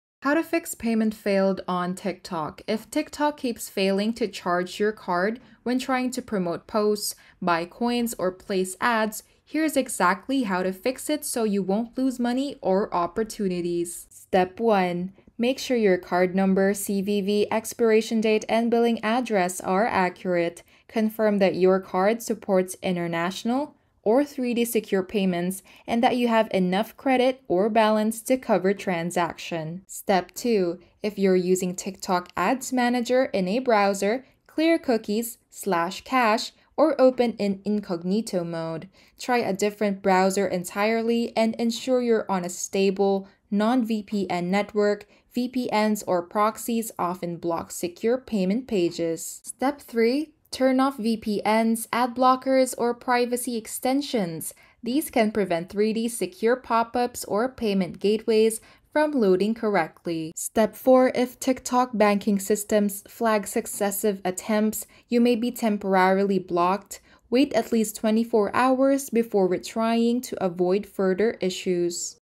How to Fix TikTok Payment Failed Error (Updated 2025)
😩 Don’t worry, in this updated 2025 tutorial, I’ll show you exactly how to fix TikTok payment errors step-by-step and get your transactions working again!